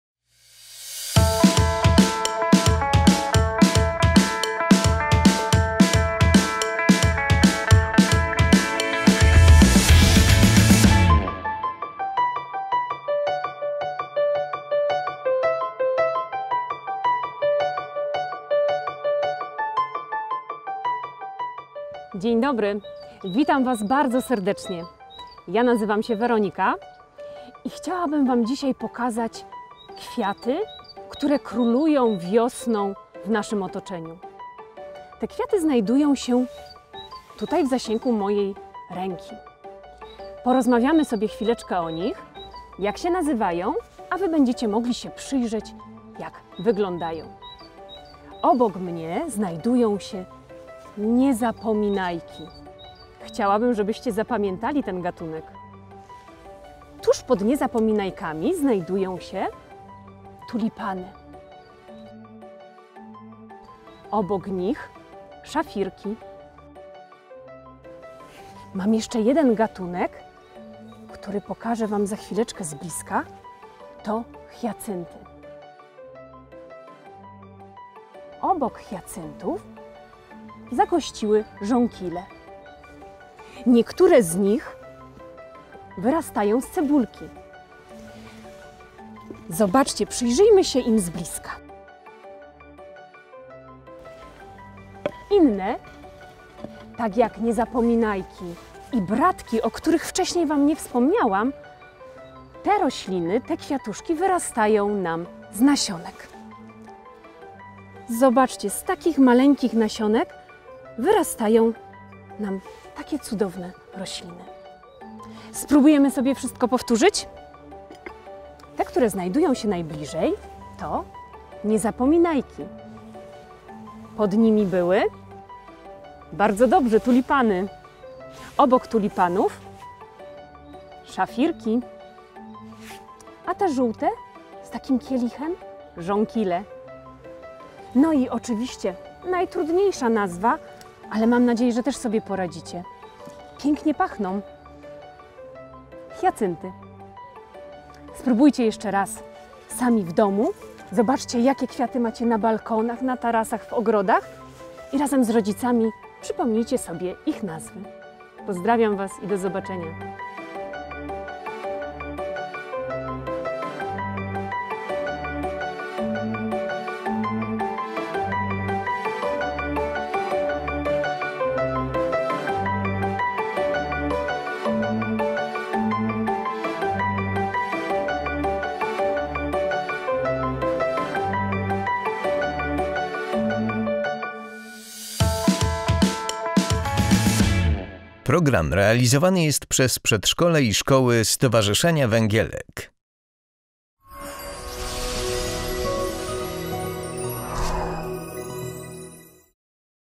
W drogę: Niezłe Ziółka / Ucz się z nami #01
Wyruszamy w drogę! Odwiedzimy zagrodę "Niezłe Ziółka" w Mikołowie, gdzie Pani Weronika pokaże nam wiosenne kwiaty.